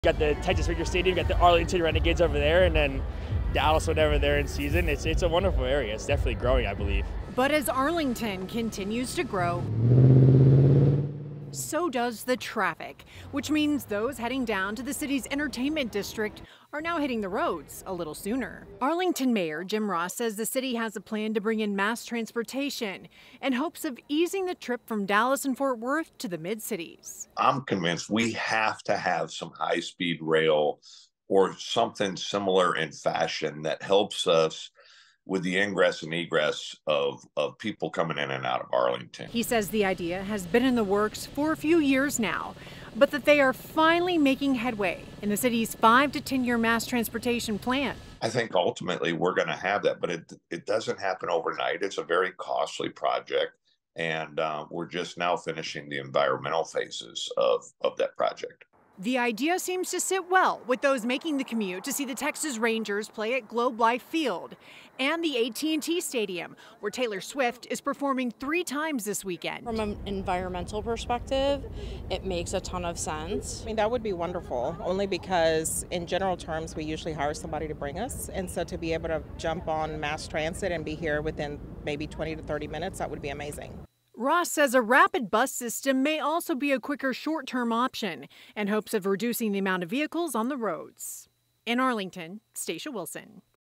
Arlington planning transit system as entertainment district grows
The city of Arlington is planning a mass transit system to ease travels from the downtown areas of Dallas and Fort Worth.

Mayor Jim Ross told WFAA the city is looking at both long-term and short-term plans.

“I’m convinced we have to have some high-speed rail or something similar in fashion that helps us with the ingress and egress of people coming in and out of Arlington,” said Ross.

The city’s entertainment district was a hot spot for both baseball and Taylor Swift fans over the weekend.

The Texas Rangers has games at Globe Life Field Saturday afternoon and Sunday evening, and music sensation Taylor Swift is performing for three nights at AT&T Stadium. The XFL's Arlington Renegades also had a game scheduled for Friday night at Choctaw Stadium.

Rangers fan Isaiah Graham-Mobley said he loves spending time in Arlington.

“It’s a wonderful area definitely growing I believe,” said Graham-Mobley.